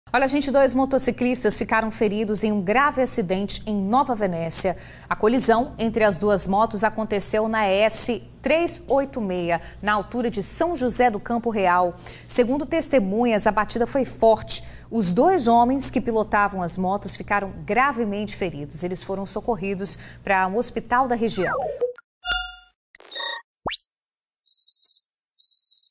Motociclistas ficam feridos em acidente no Noroeste do ES
A colisão entre as motos ocorreu na BR-386, no município de Nova Venécia.